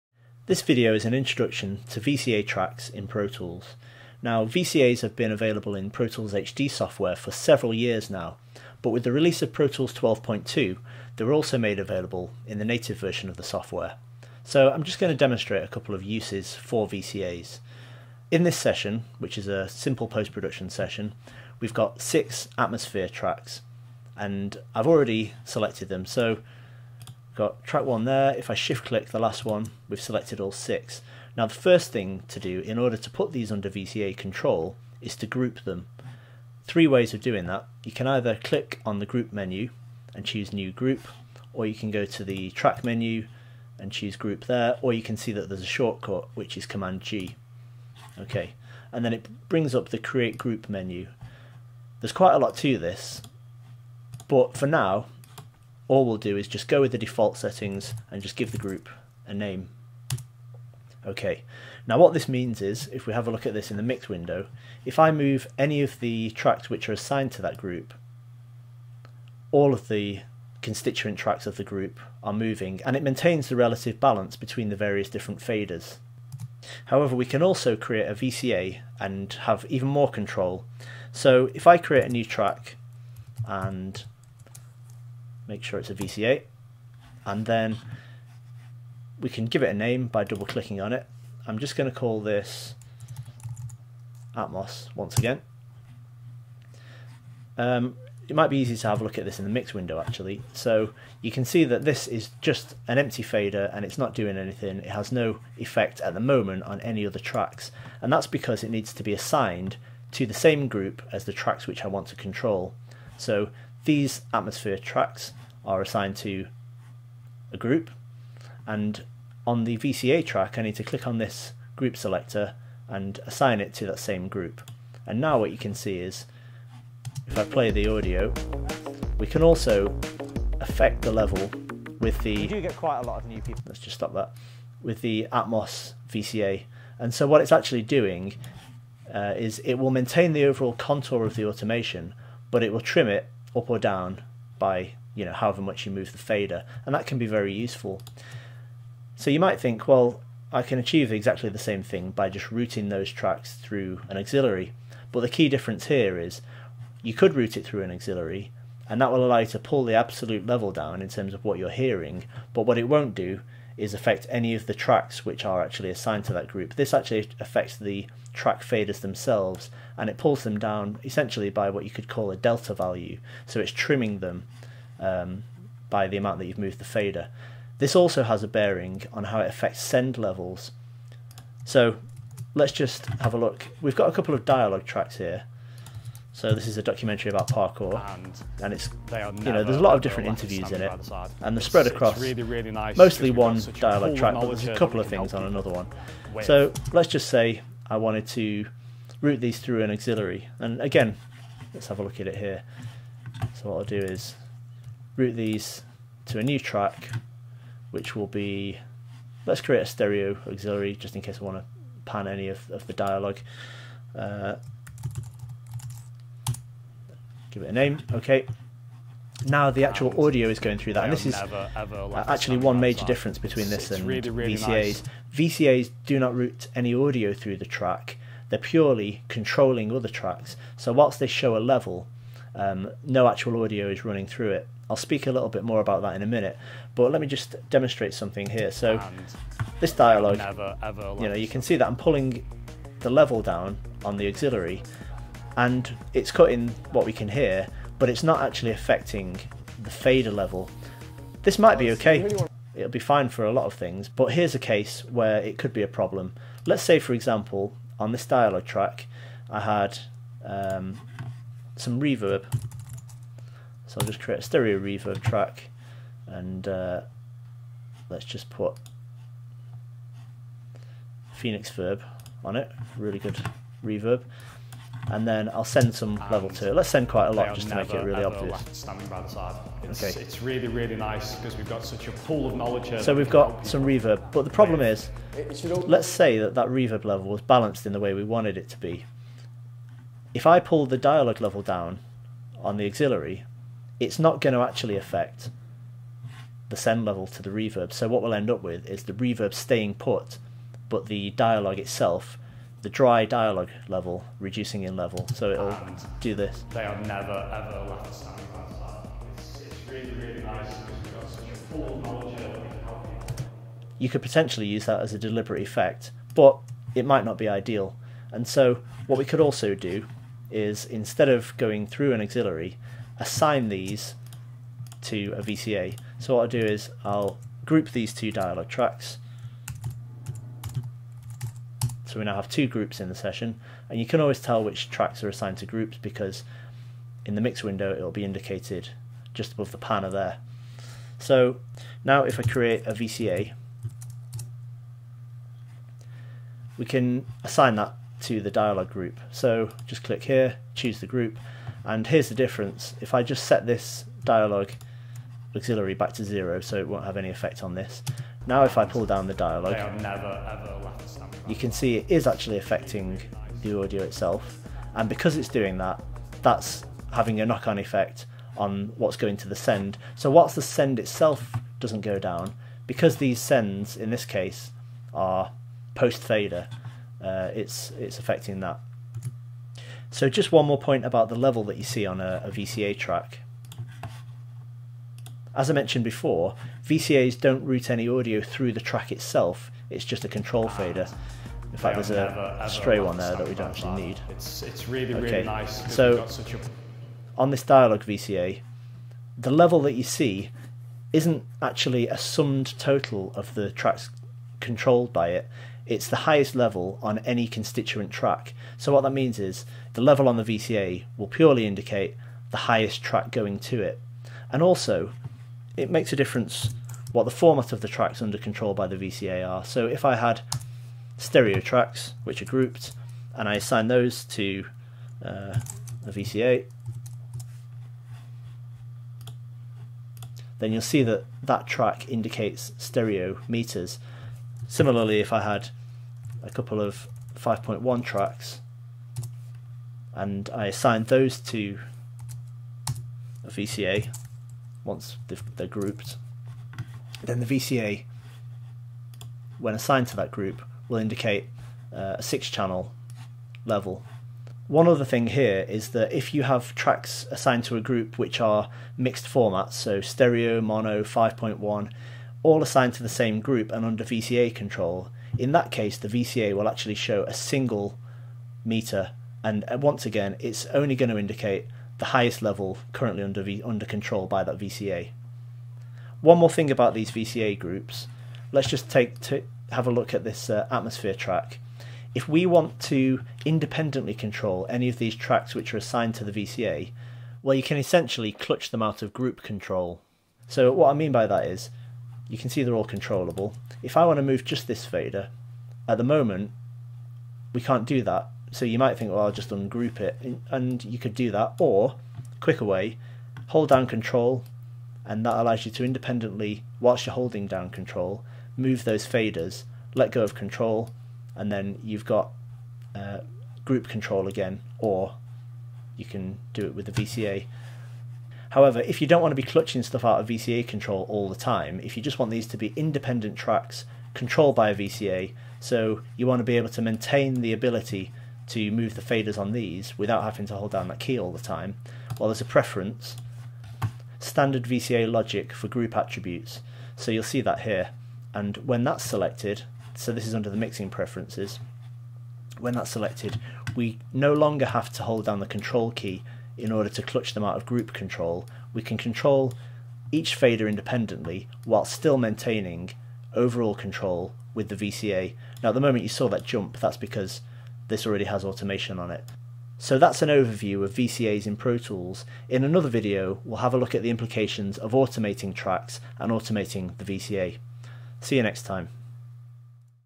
Introduction to VCAs in Pro Tools
VCA masters have been available in Pro Tools HD software for a number of years. As of Pro Tools 12.2 they are also available in the native version of Pro Tools. A VCA (Voltage Controlled Amplifier) provides a means of easily controlling a group of faders, allowing the overall level of the grouped tracks to be brought up or down whilst maintaining the relative balance of the group members and retaining the individual track automation. This video gives an overview of how to setup and use VCAs and how they differ from aux tracks.